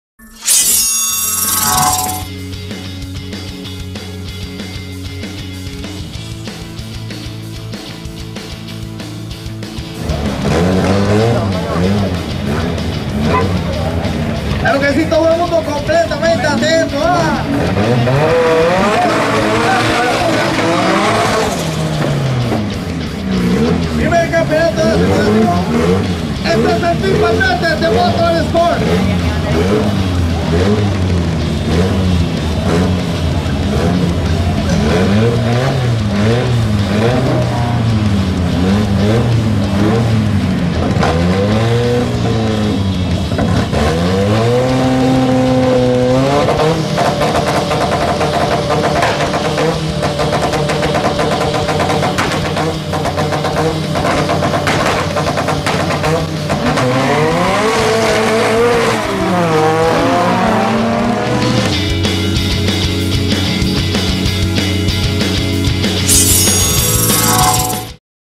PROMO DRAG COSTA RICA Team Valverde Motor Sport
Video promocional del 1er Campeonato de Aceleración del Team Valverde Motor Sport.